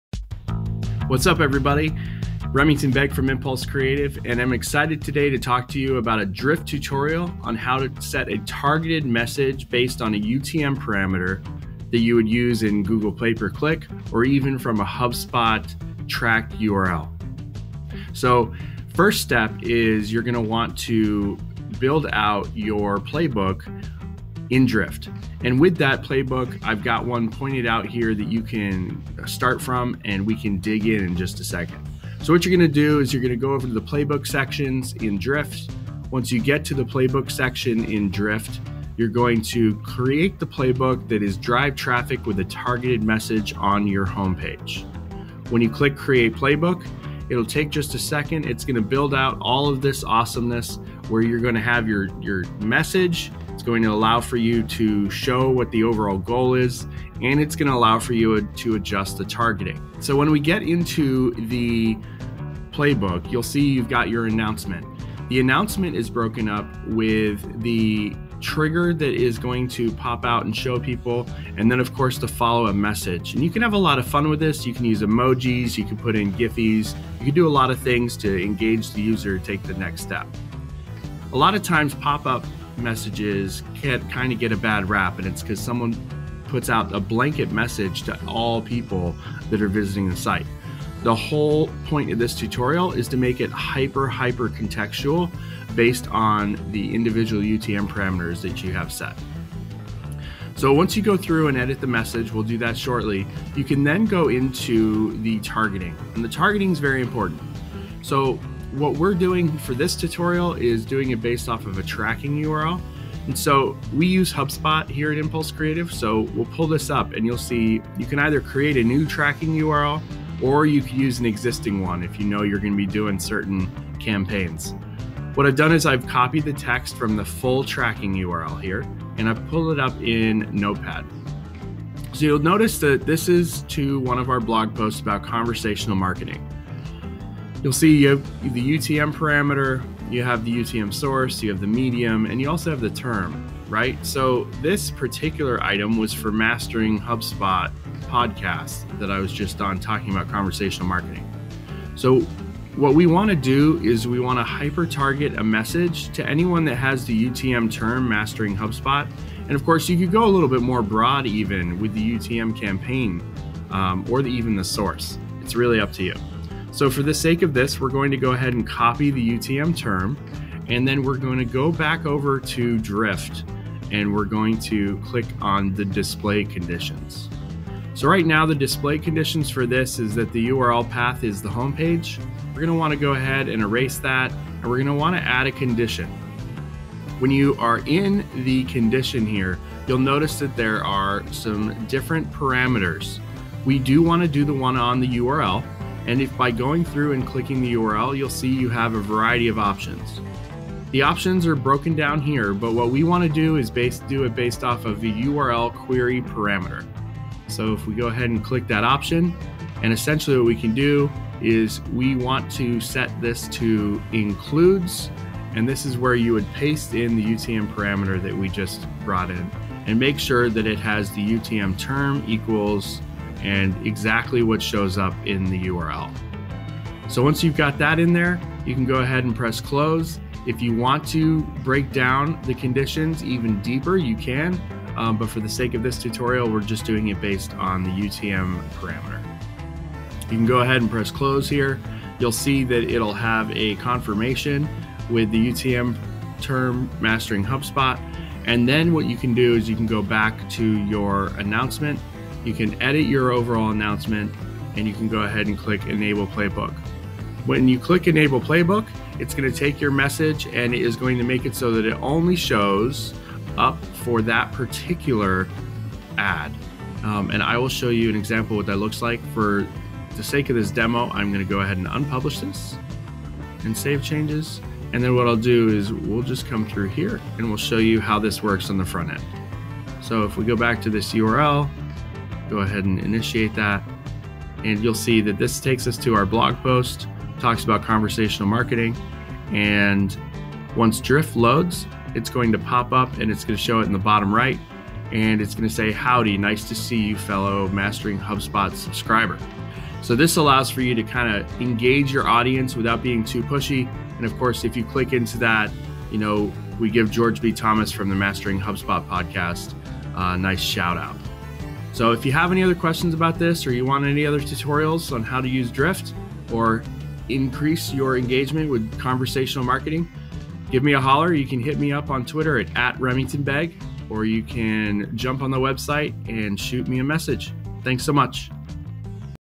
Drift Tutorial: Setting up Drift Chat to trigger based on utm parameter
I'm excited today to talk to you about a drift tutorial on how to set a targeted message based on a utm parameter that you would use in Google Pay Per click, or even from a hubspot tracking url.
So first step is you're going to want to build out your playbook in drift, and with that playbook, I've got one pointed out here that you can start from and we can dig in just a second. So what you're gonna do is you're going to go into the playbook sections in Drift. Once you get to the playbook section in Drift, you're going to create the playbook that is "Drive traffic with a targeted message on your homepage".
When you click create playbook, it'll take just a second. It's going to build out all of this awesomeness where you're gonna have your message. It's going to allow for you to show what the overall goal is and it's going to allow for you to adjust the targeting.
So when we get into the playbook, you'll see you've got your announcement. The announcement is broken up with the trigger that is going to pop out and show people and then of course the follow-up message and you can have a lot of fun with this. You can use emojis, you can put in Giphys, you can do a lot of things to engage the user, take the next step.
A lot of times pop up messages and kind of get a bad rap and it's because someone puts out a blanket message to all people that are visiting the site. The whole point of this tutorial is to make it hyper, hyper contextually based on the individual utm parameters that you have set.
So what we're doing for this tutorial is doing it based off of a tracking url,  we use Hubspot here at Impulse Creative. So we'll pull this up and you'll see you can either create a new tracking url or you've used an existing one. If you know you're going to be doing certain campaigns. What I've done is I've copied the text from the full tracking url here and I pull it up in notepad, so you'll notice that this is one of our blog posts about conversational marketing. You'll see a utm parameter. You have the UTM source, you have the medium, and you also have the term right. So this particular item was for Mastering Hubspot podcasts that I was just on talking about conversational marketing. So what we want to do is we want to hyper target a message to anyone that has the utm term mastering hubspot.
And of course you go a little bit more broad even with utm campaign, even the source. It's really up to you. So for the sake of this tutorial, we're going to go ahead and copy the utm term and then we're going to go back over to Drift. And we're going to click on the display conditions. So right now the display conditions for this is that the url path is the homepage. We want to go ahead and erase that and we're going to want to add another condition. When you are in the condition here, you'll notice that there are some different parameters. We do want to do the one on the url and if by going through and clicking the drop down, you'll see you have a variety of options. The options are broken down here,  what we want to do is basically do it based off of the url query parameter, so if we go ahead and click that option and essentially what we can do is we want to set this to includes, and this is where you would paste in the utm parameter that we just brought in and make sure that it has the utm term equals and exactly what shows up in the url.